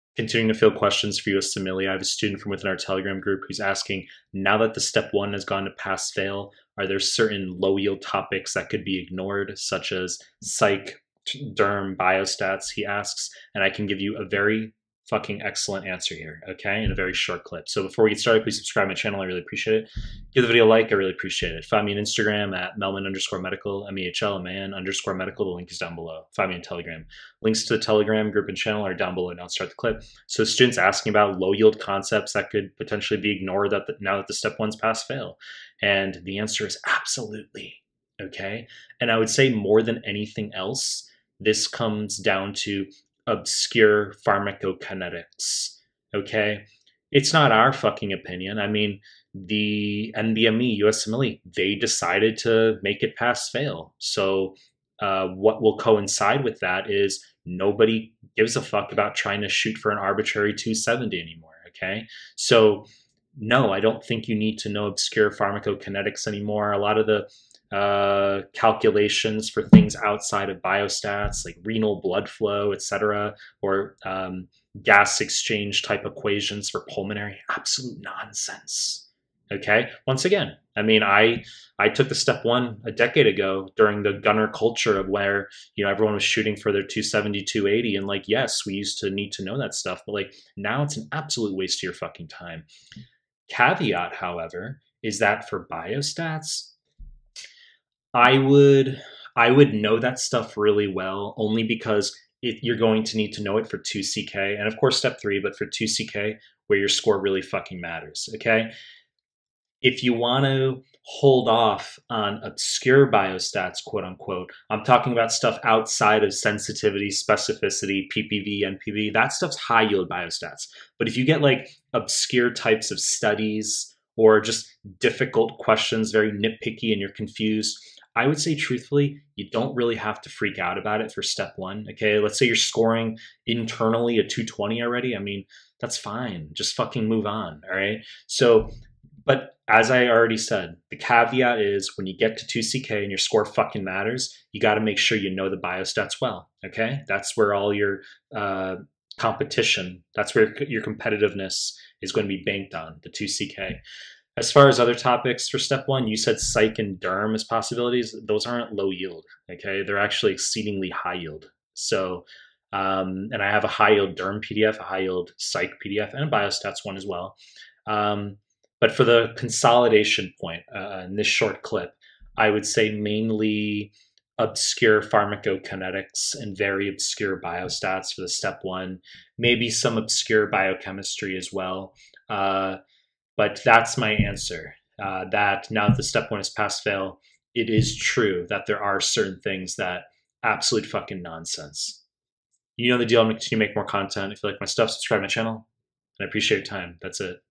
"Now that Step 1 is P/F, are there low-yield topics I can ignore?"
In this clip I address a student's question about potential low-yield topics that could be ignored now that Step 1 has gone to pass/fail.